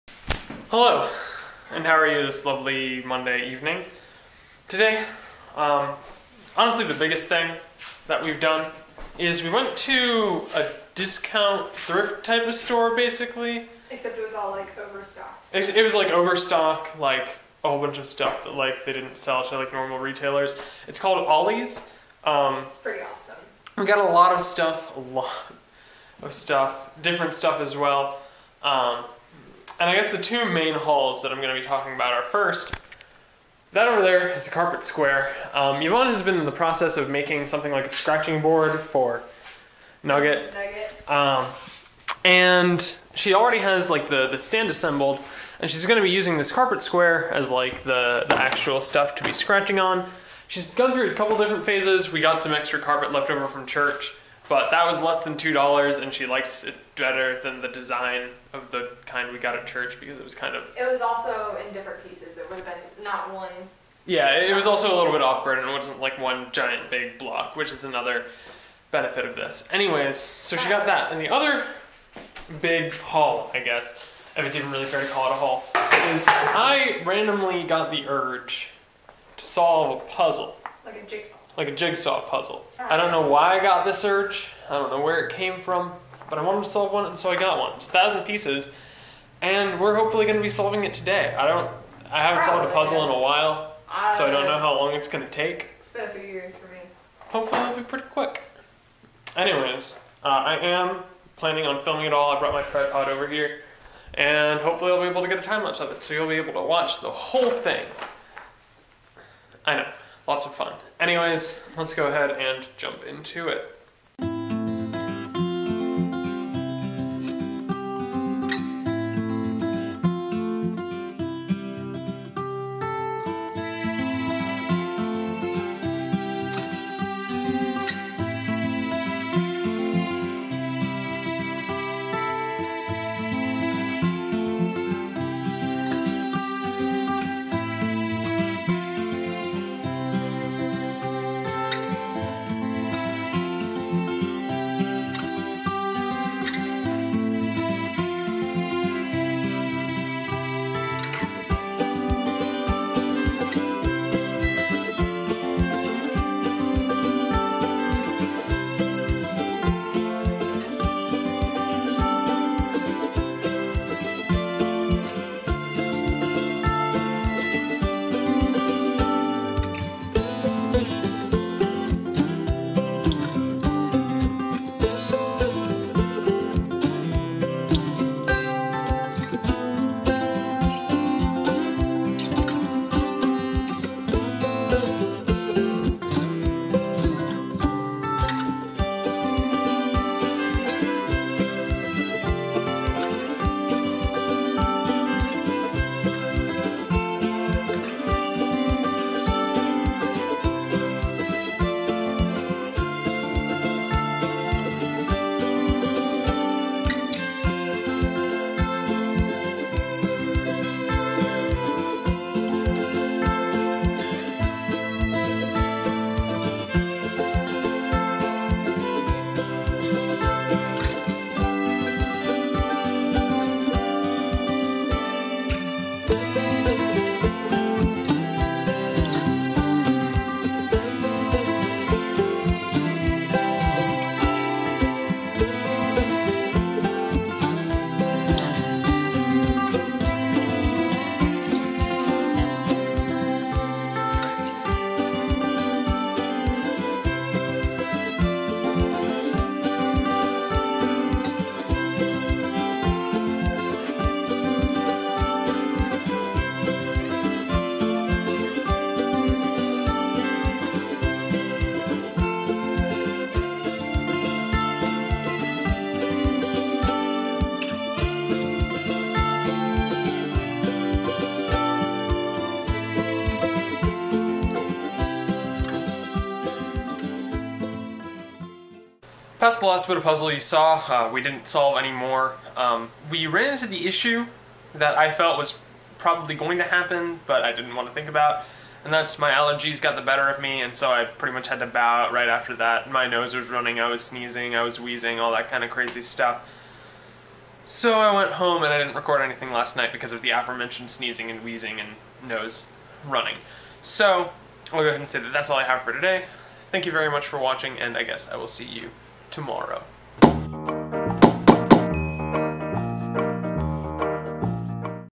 Jigsaw Puzzle | Day 284 (10/10/16)
An impromptu shopping trip to Ollie's, and a fun evening activity.